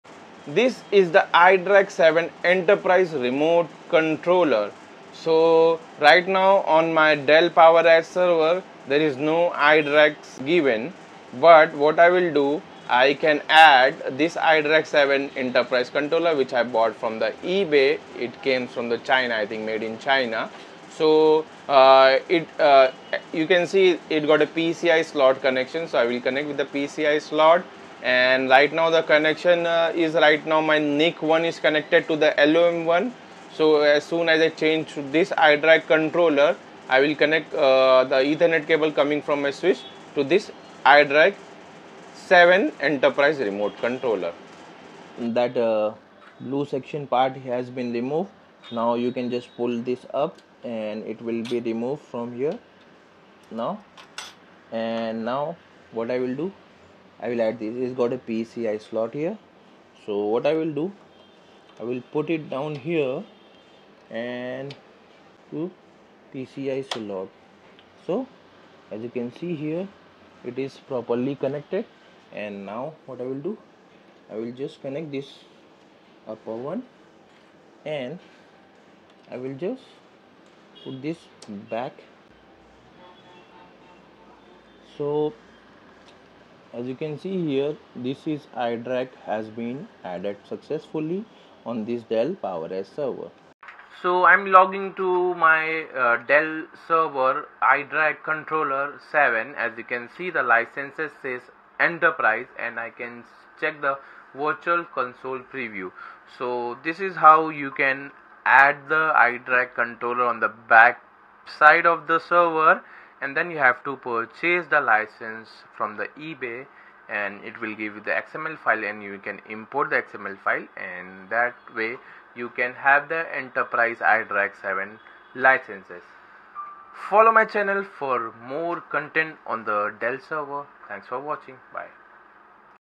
How I Replaced iDRAC7 in My Dell R420
In this short video, I quickly show you how I replaced the iDRAC7 controller on my Dell PowerEdge R420 server. If your iDRAC is unresponsive, stuck, or not booting, this might help you fix it!

✅ Steps covered:
 • Opening the server
 • Locating the iDRAC7 controller
 • Safe removal & replacement
 • Power-up check

🔧 Works for most Dell 12th Gen PowerEdge models.